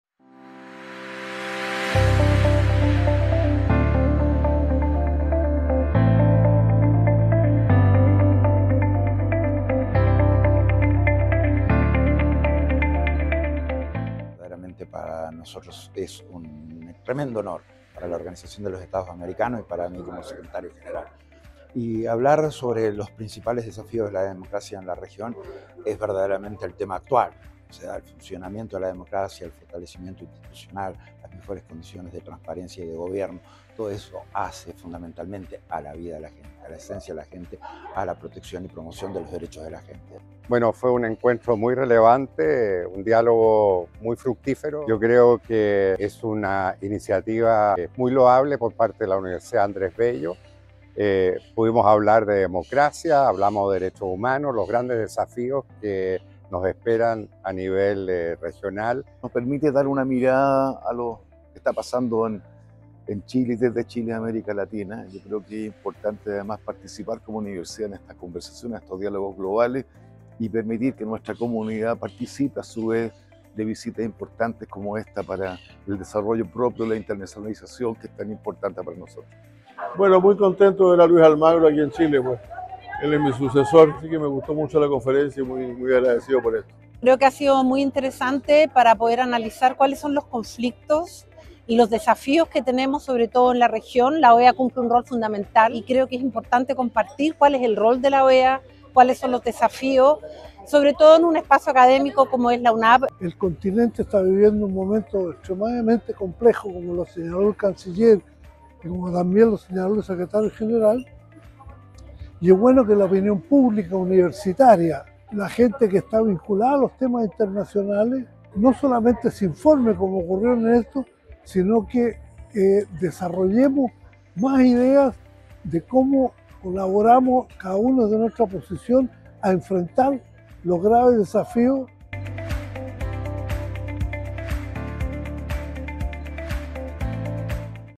Conversatorio "Diálogos Globales: Desafíos y oportunidades para Chile y América"
La segunda edición del conversatorio "Diálogos Globales" analizó los desafíos y oportunidades para Chile y América.

Esta instancia contó con la destacada participación del secretario general de la Organización de los Estados Americanos (OEA), Luis Almagro y del ministro de Relaciones Exteriores, Alberto van Kleveren.